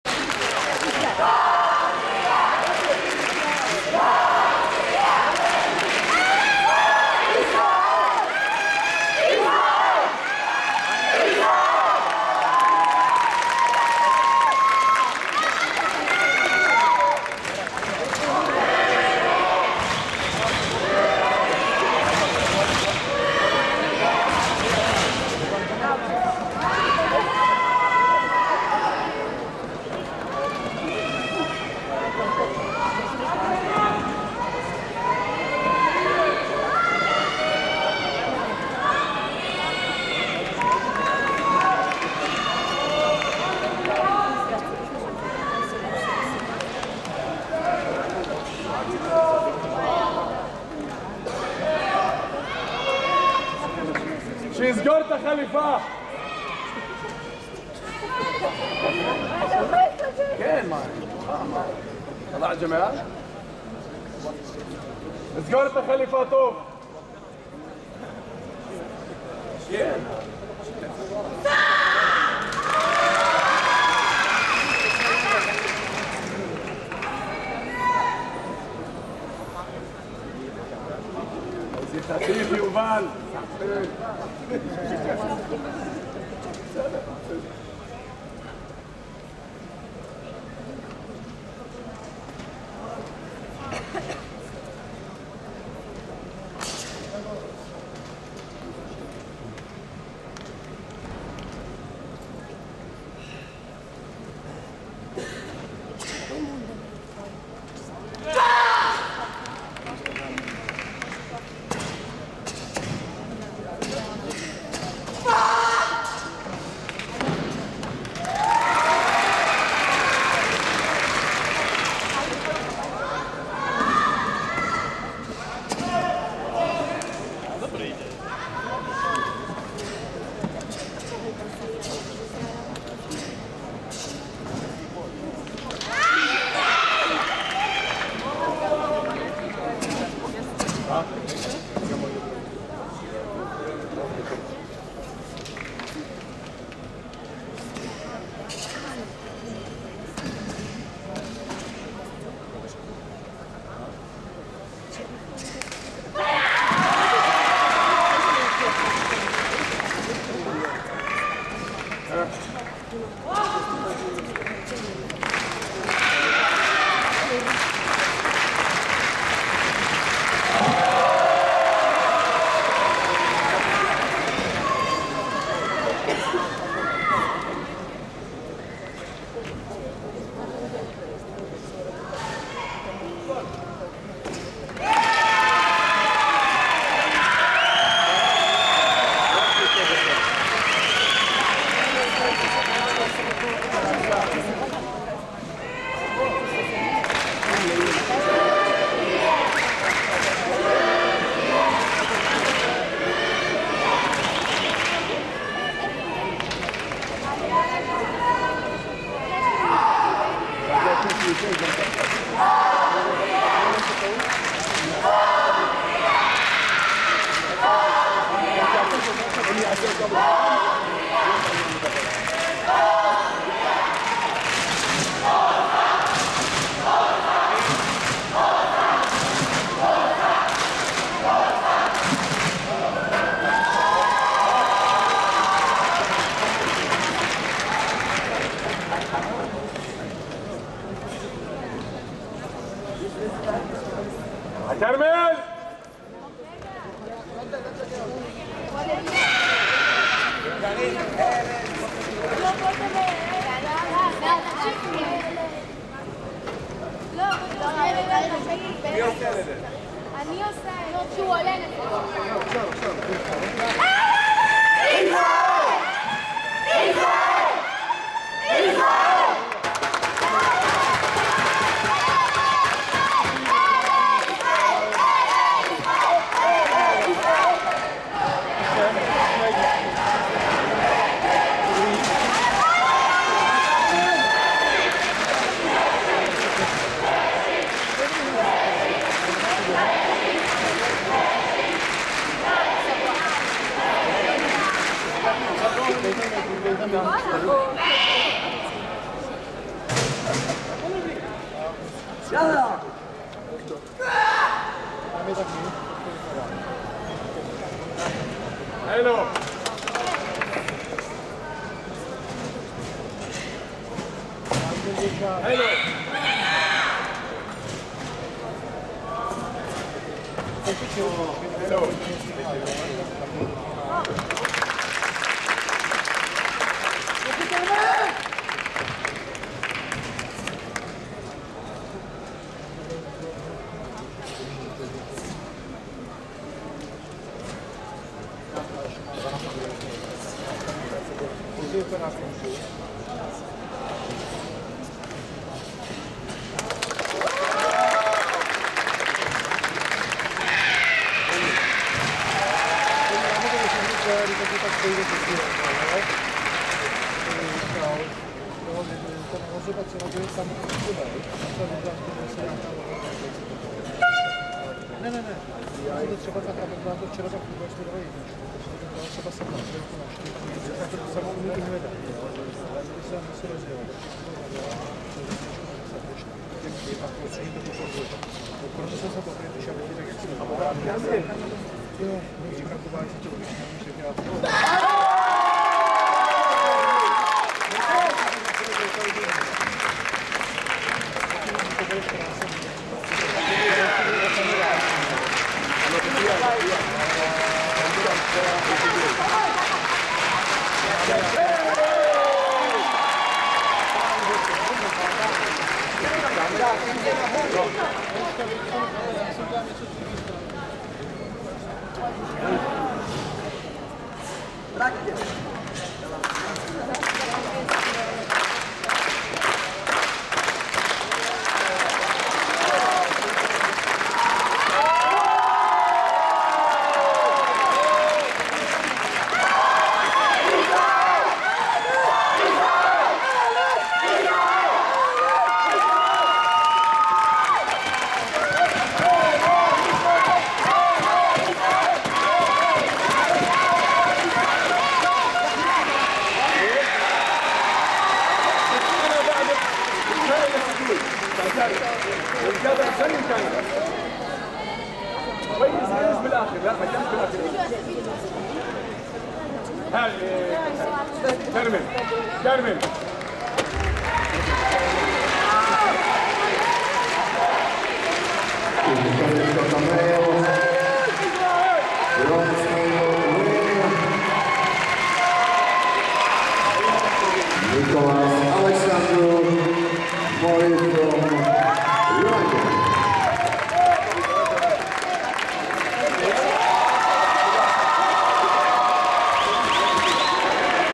EKF Junior, Cadet & U21 Championships 2022 Prague, Cadet Kata Male Bronze Medal Match: MODI : MANSOR
EKF Junior, Cadet & U21 Championships 2022 Prague
Cadet Kata Male Bronze Medal Match:
MODI ALEXANDRU NICOLAS (ROU) KANKU SHO 24.98
MANSOR CARMEL (ISR) GANKAKU 23.48